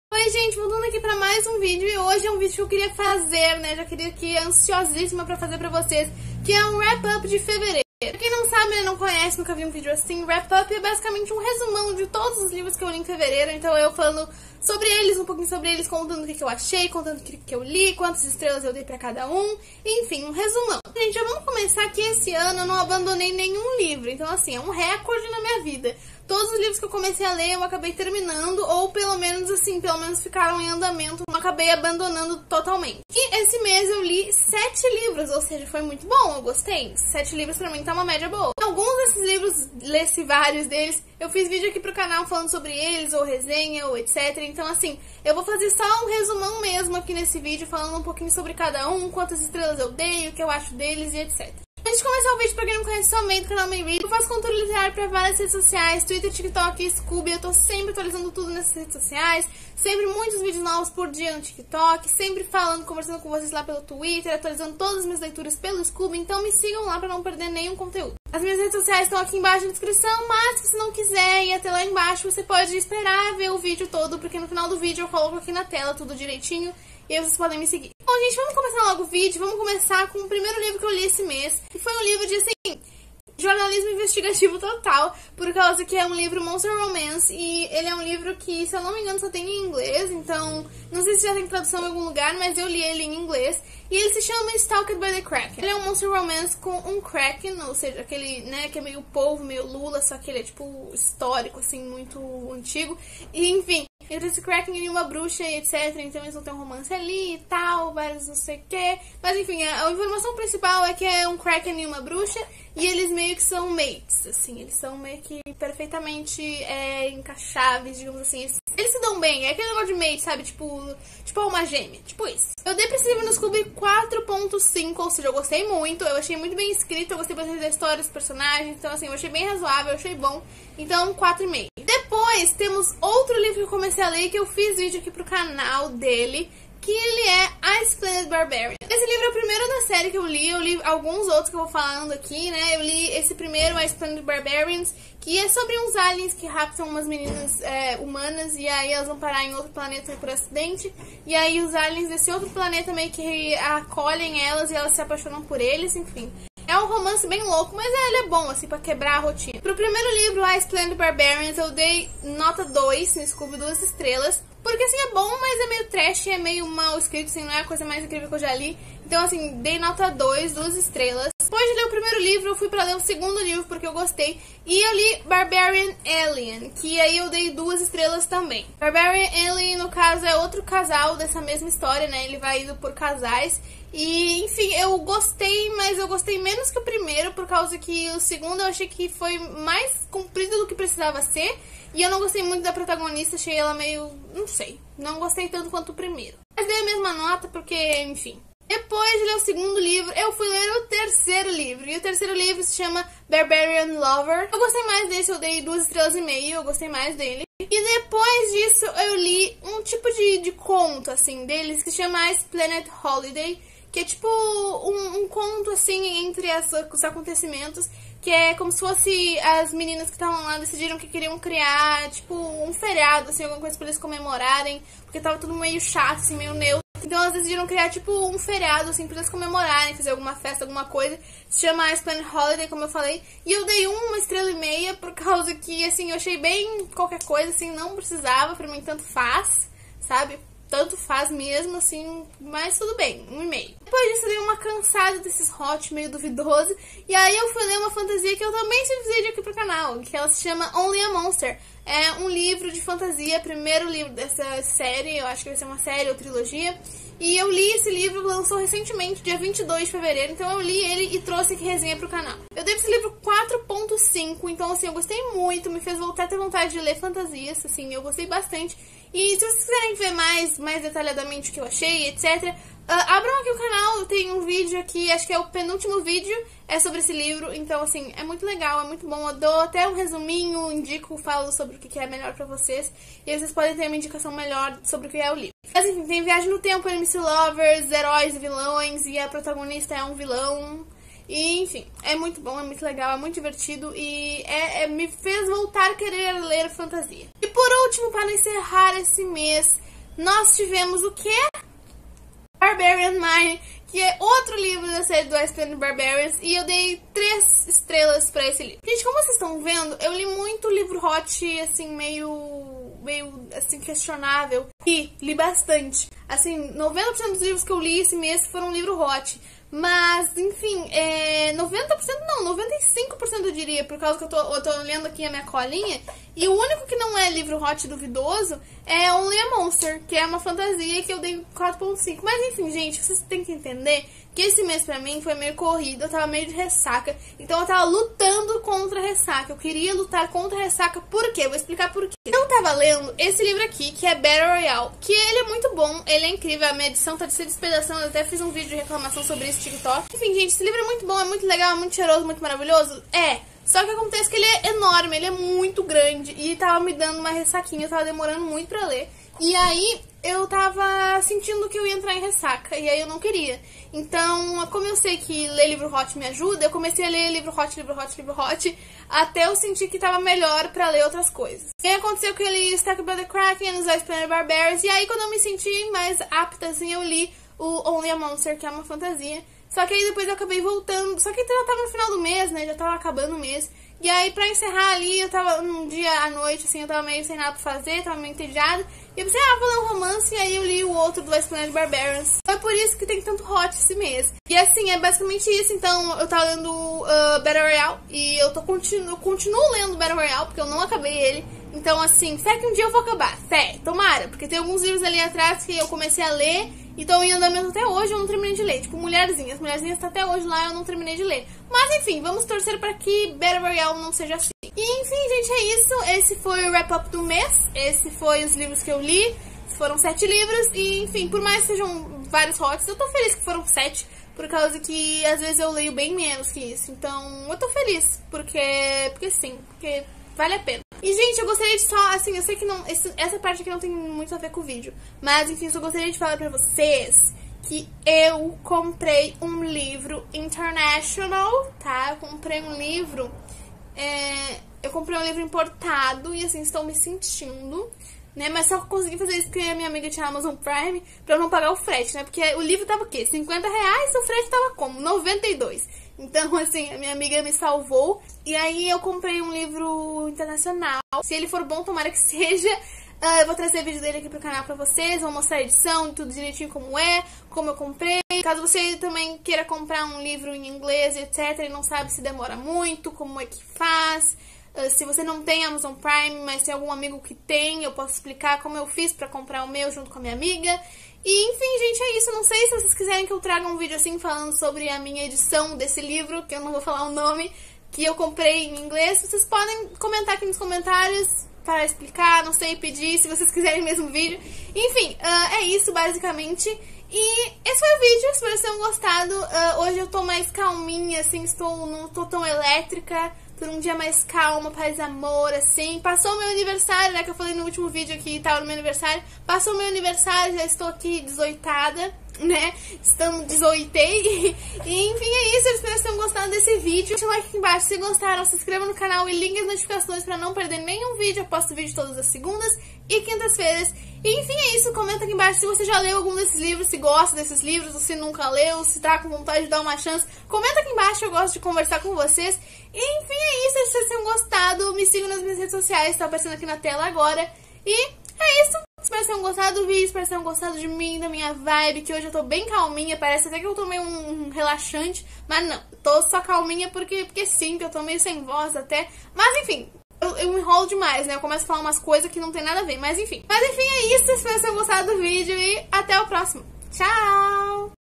WRAP UP DE FEVEREIRO! Muito livro bom e vários monster romances 💐 | May Reads!
Oioi! No vídeo de hoje eu fiz um wrap up (resumo) de todos os livros que li no mês de fevereiro! Teve muita coisa boa e muito monster romance, não consegui me segurar.
Qualquer pergunta deixe aqui nos comentários e ficarei muito feliz em responder 💙

Não se esqueça de assistir o vídeo na maior qualidade possível para uma melhor experiência.

💙 Dá um like
💙 Compartilhe

• wishlist da amazon

Obrigada por ler a descrição ✨